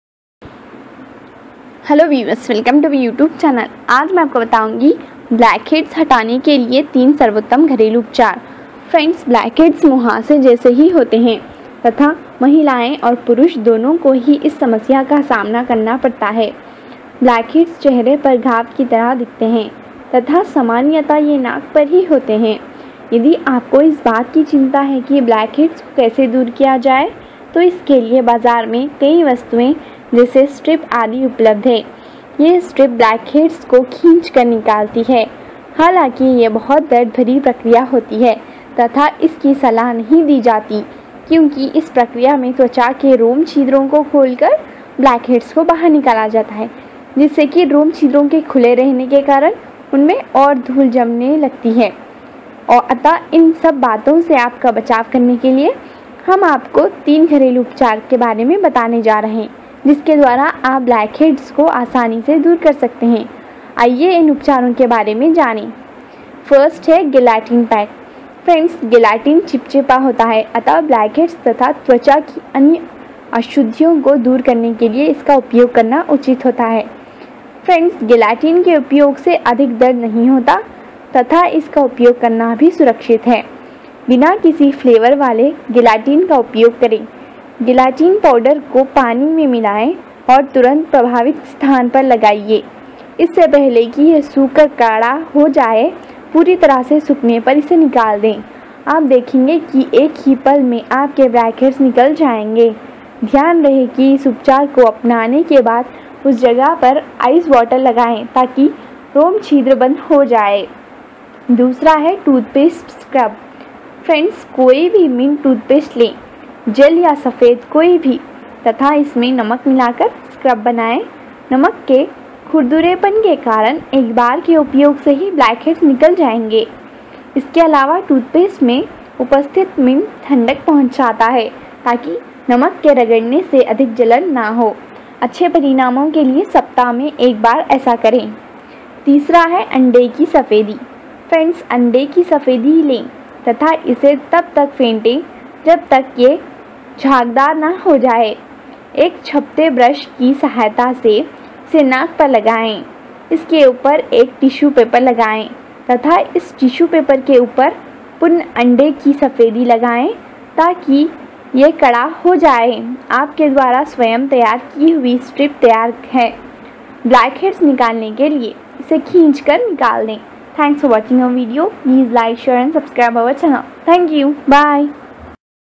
3 सर्वोत्तम घरेलू उपचार ब्लैक हैड्स हटाने के लिए | How to Remove Blackheads From Nose & Face
ब्लैक हैड्स हटाने के लिए - 3 सर्वोत्तम घरेलू उपचार ब्लैक हैड्स हटाने के लिए | How to Remove Blackheads From Nose & Face. read more @

जिलेटिन पैक: जिलेटिन चिपचिपा होता है अत: ब्लैक हैड्स तथा त्वचा की अन्य अशुद्धियों को दूर करने के लिए इसका उपयोग करना उचित होता है। जिलेटिन के उपयोग से अधिक दर्द नहीं होता तथा इसका उपयोग करना भी सुरक्षित है। बिना किसी फ्लेवर वाले जिलेटिन का उपयोग करें। जिलेटिन पाउडर को पानी में मिलाएं और तुरंत प्रभावित स्थान पर लगायें इससे पहले कि यह सूखकर कड़ा हो जाए। पूरी तरह सूखने पर इसे निकाल दें। आप देखेंगे कि एक ही पल में आपके ब्लैक हैड्स निकल जायेंगे। ध्यान रहे कि इस उपचार को अपनाने के बाद उस जगह पर आइस वॉटर लगायें ताकि रोम छिद्र बंद हो जाएँ।

टूथपेस्ट स्क्रब: कोई भी मिंट टूथपेस्ट लें (जेल या सफ़ेद कोई भी) तथा इसमें नमक मिलाकर स्क्रब बनायें। नमक के खुरदुरेपन के कारण एक बार के उपयोग से ही ब्लैक हैड्स निकल जायेंगे। इसके अलावा टूथपेस्ट में उपस्थित मिंट ठंडक पहुंचाता है ताकि नमक के रगड़ने से अधिक जलन न हो। अच्छे परिणामों के लिए सप्ताह में एक बार ऐसा करें।

अंडे की सफ़ेदी: अंडे की सफ़ेदी लें तथा इसे तब तक फेंटे जब तक यह झागदार न हो जाए। एक छपते ब्रश की सहायता से इसे नाक पर लगायें। इसके ऊपर एक टिशु पेपर लगायें तथा इस टिशु पेपर के ऊपर पुन: अंडे की सफ़ेदी लगायें ताकि यह कड़ा हो जाए। आपके द्वारा स्वयं तैयार की हुई स्ट्रिप तैयार है। ब्लैक हैड्स निकालने के लिए इसे खींचकर निकाल दें।